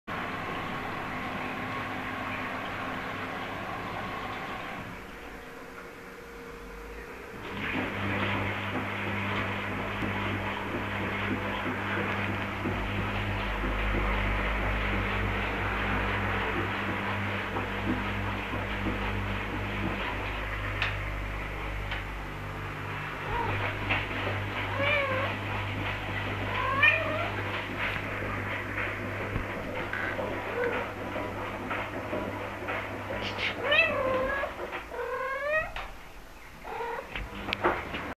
Skarpetka i pralka
Jakby pierwszy raz na oczy pralke widziała. no ale cóż, dziwna to kicia :D
Przepraszam za bałagan, było to w trakcie porządków ;)